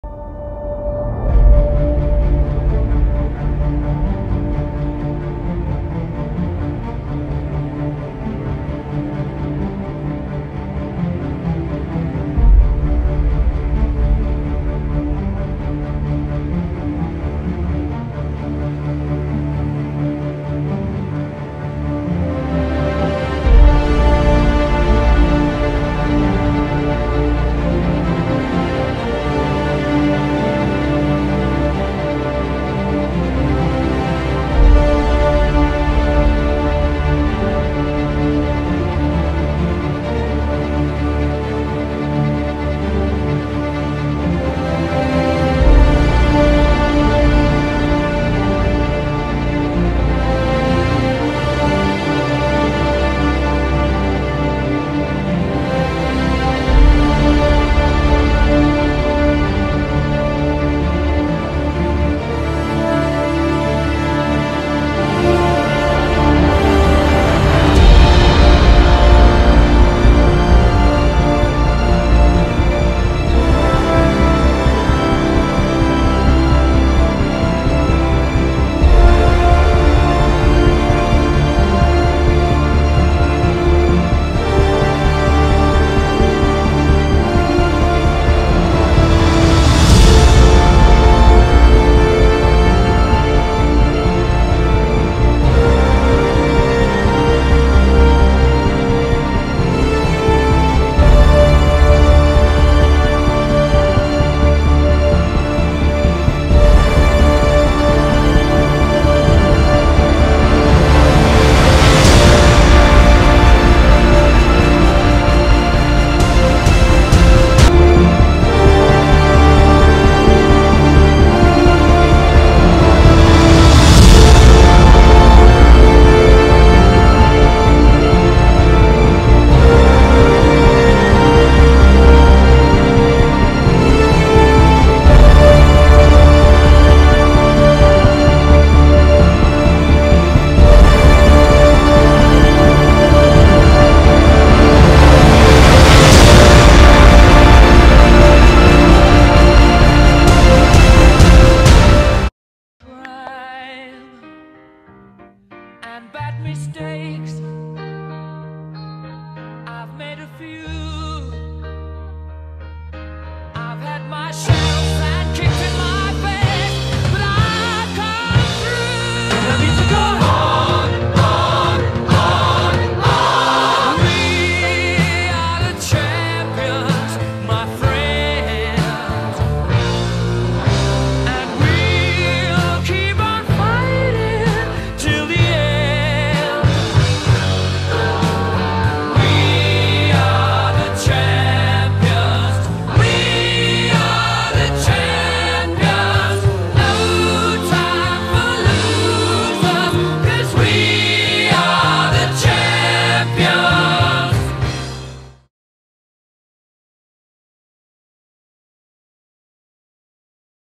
CSKA Moscow 4 - 1 Metallurg Magnitagorsk Play-off 21./22. (7 Final Game )
Gagarin Cup 2021./2022.
(0:1)Pavel Karnaukhov (Konstantit Okulov)
(0:2)Alexander Popov (Andrei Svetlakov)
(0:3)Konstantin Okulov (Mikhail Grigorenko,Pavel Karnaukhov)
(1:3)Philippe Maillet (Andrei Chibisov)
(1:4)Vladislav Kamenev

CSKA Moscow Goalie:Ivan Fedotov
Metallurg Magnitagosk Goalie:Vaily Koshechkin